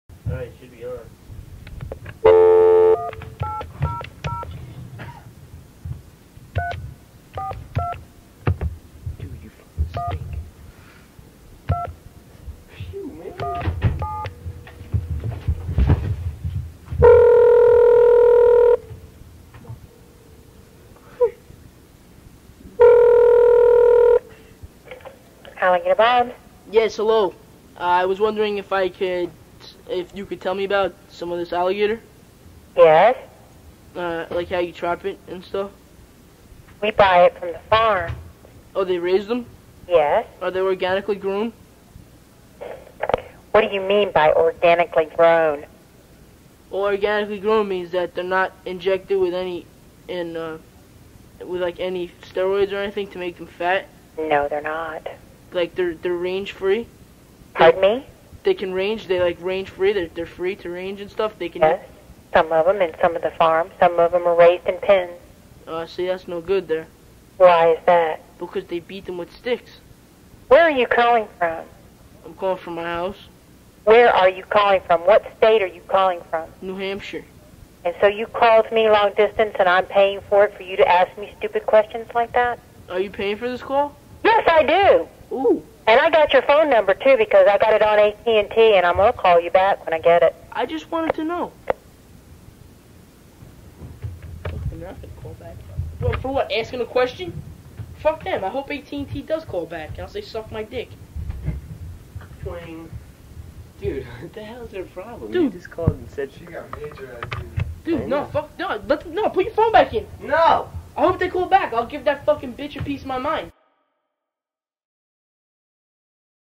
Muffin Asses - "Alligator Bob's" [Prank call] with James Geraghty 1994
Muffin Asses - "Aligator Bob's" [Prank call] 1994.

The Muffin Asses were a group of high school kids who made prank calls and did retarded shit for fun. On this particular call, James didn't use a character voice and was just asking general questions, I'm not sure why he decided not to fuck with her but he didn't. We always wondered why the lady was such a cunt. :)